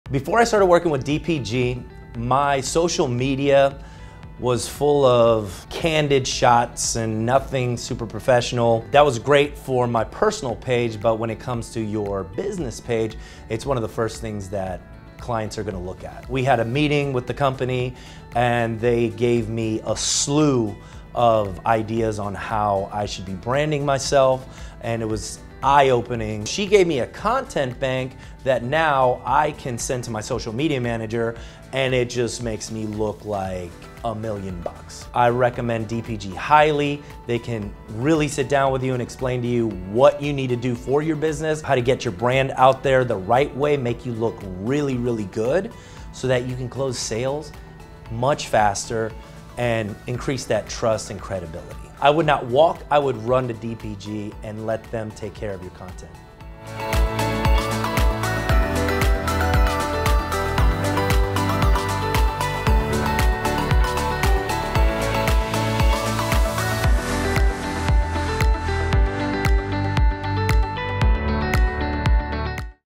Davillier Photography and Graphics - Armando Leduc Testimonial
Armando Leduc, Founder, Producer, and Filmmaker of Leduc Entertainment, discusses his experience with Davillier Photography and Graphics!